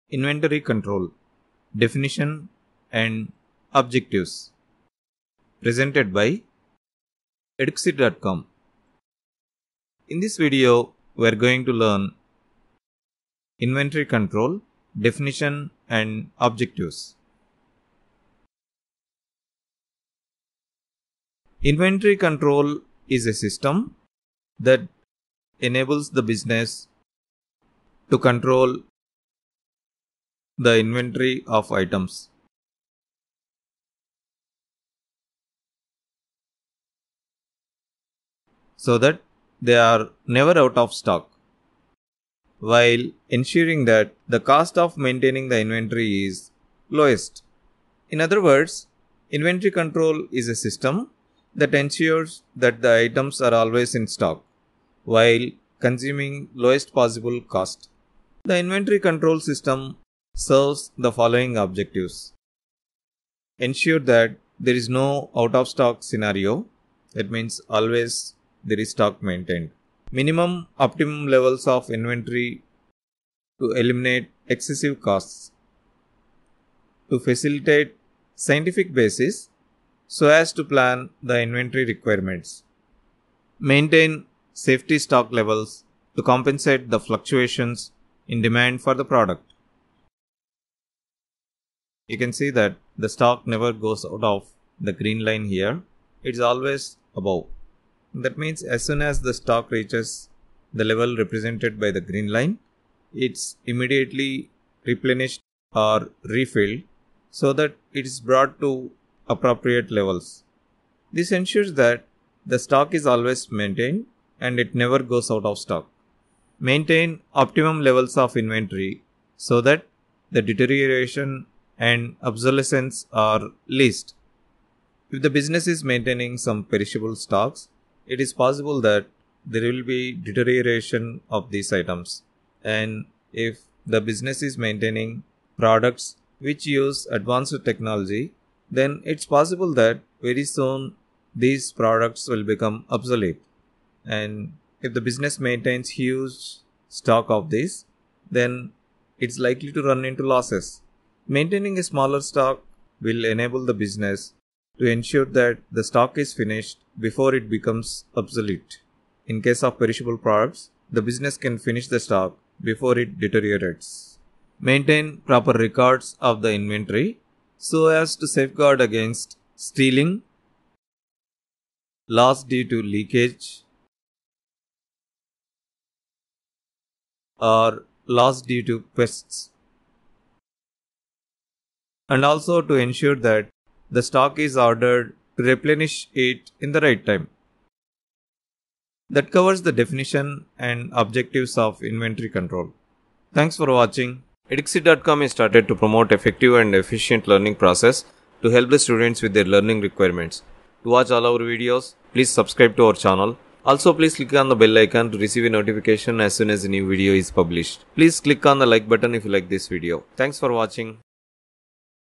Inventory Control - Definition and Objectives
What is inventory control?

Inventory control is a system that enables the businesses to control the inventory of items so that they are never out of stock, while ensuring that the cost of maintaining the inventory is lowest. In other-words inventory control is a system that ensures that the items are always in stock while consuming lowest possible cost.

What primary objectives does the inventory control system serves?

The inventory control systems serves the following objectives.
    - Ensure that there is no out of stock scenario.
    - Maintain optimum levels of inventory te eliminate excessive costs.
    - To facilitate scientific basis so as to plan the inventory requirements.
    - Maintain safety stock levels to compensate the fluctuations in demand for the product.
    - Maintain optimum levels of inventory so that the deterioration and obsolescence are least.
    - Maintain proper records of the inventory so as to safeguard against stealing, loss due to leakage or pests and also to ensure that the stock is ordered to replenish it in the right time.